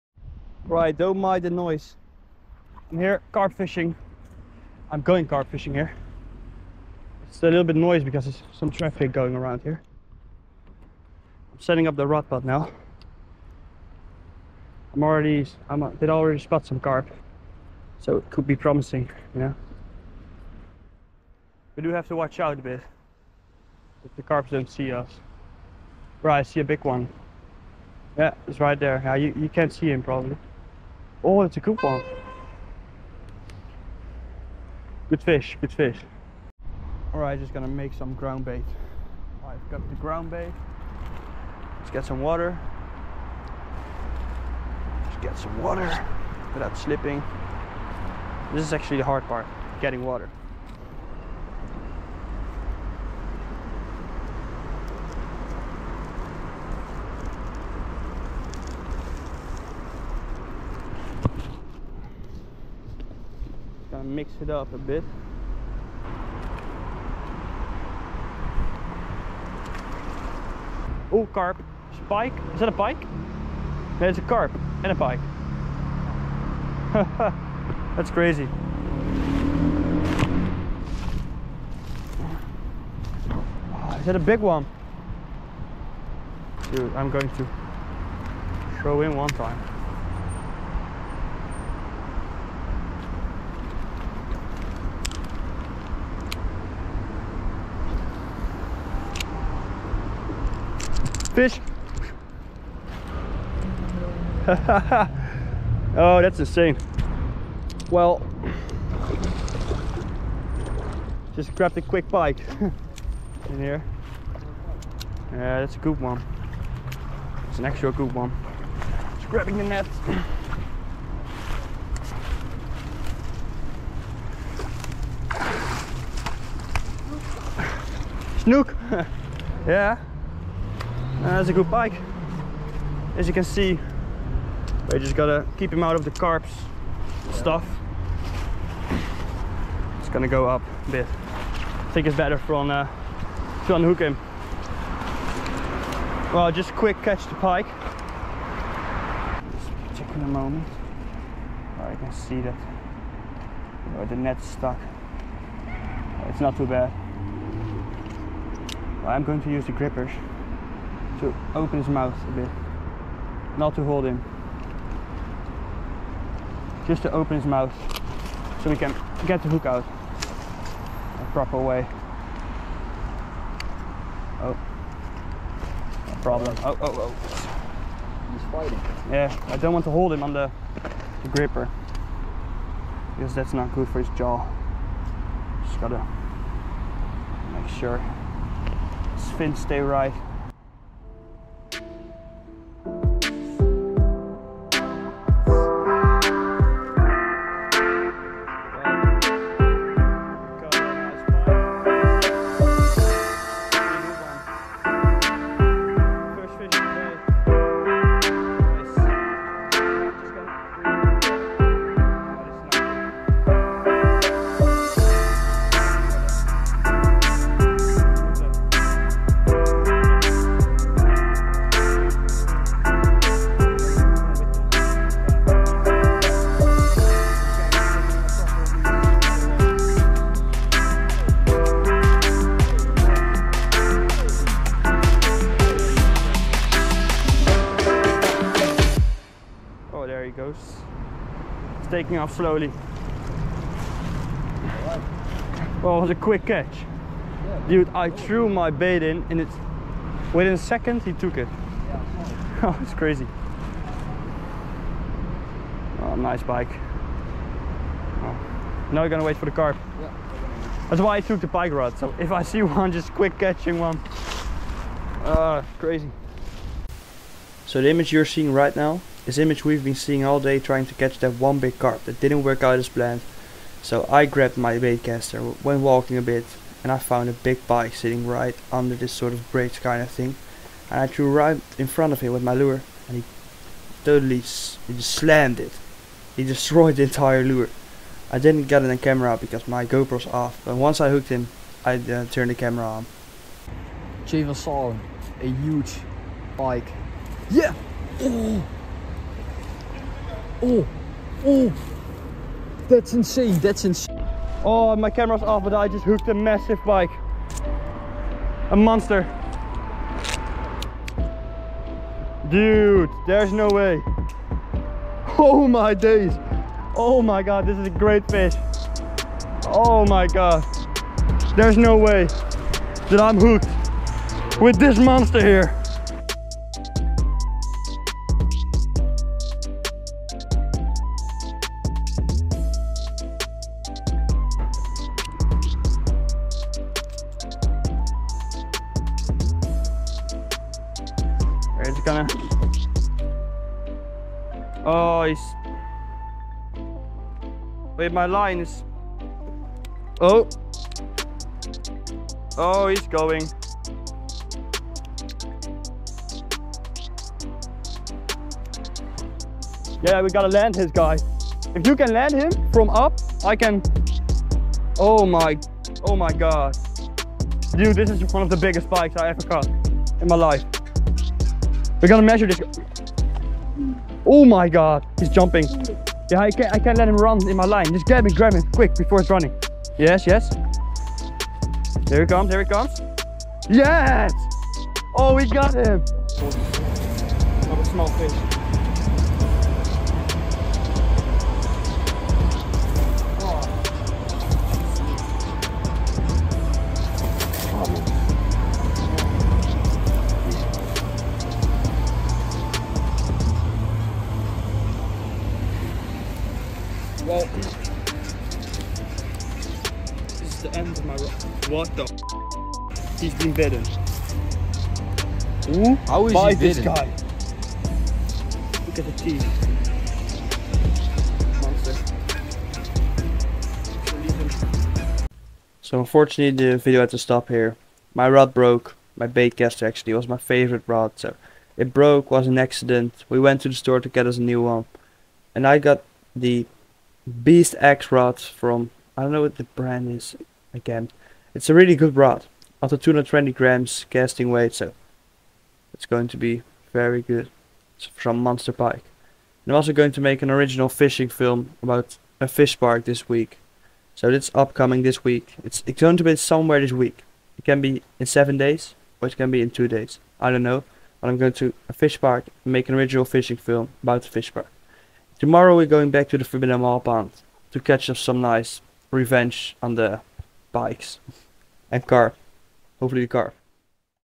Unexpected MONSTER while carp fishing (broken rod)
Today i went carp fishing with seth from @ASE_fishing  but everything changed when we caught some massive fish. And my rod broke.